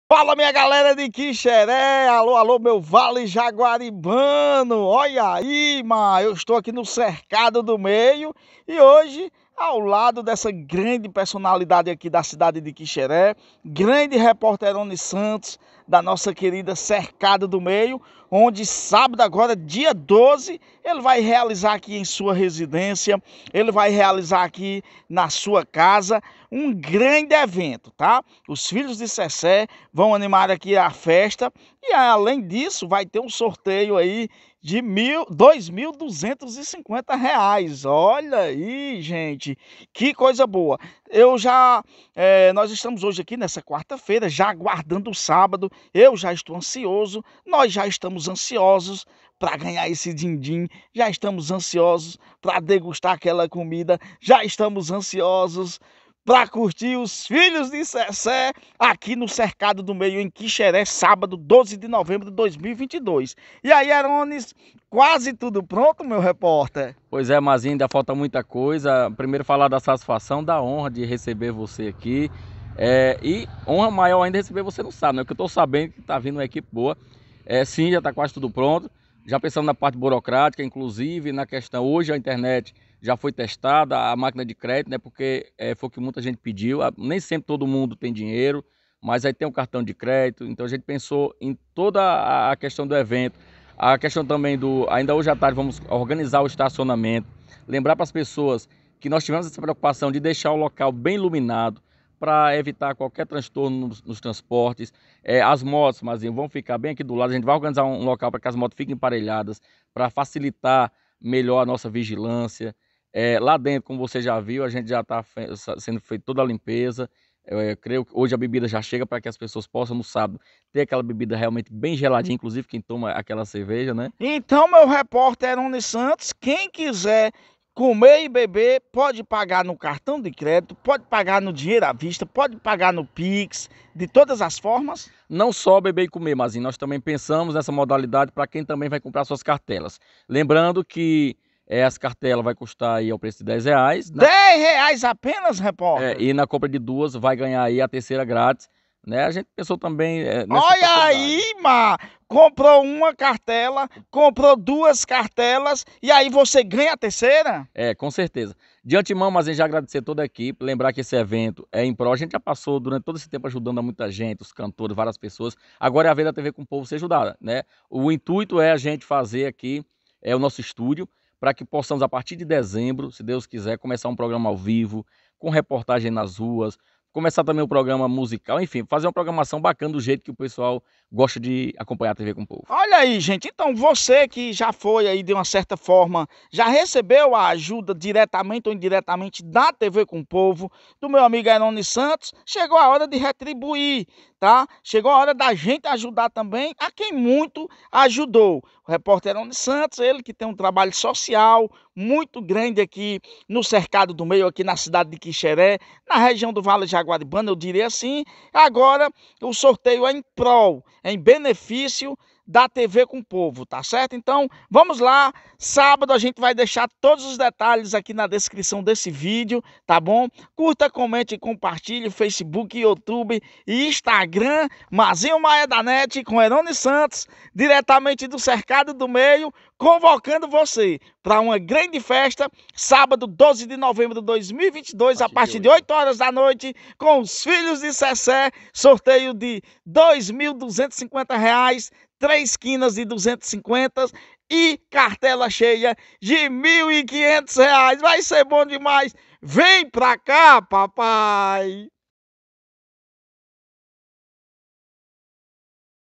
ATENÇÃO QUIXERÉ Neste sábado dia 12/11/2022 na comunidade do Cercado do Meio, Residência de Erones.
ATENÇÃO QUIXERÉ & REGIÃO VIZINHAS
Neste sábado dia 12 de novembro 2022 na comunidade do Cercado do Meio, haverá um sorteio beneficente de R$ 2.250 em prol da TV COM O POVO do repórter ERONES SANTOS.

A festa vai ficar por conta dos FILHOS de CECÉ
Cartela apenas R$ 10,00
Comprando duas ganha a terceira. Informações  ☎️ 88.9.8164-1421 Erones Santos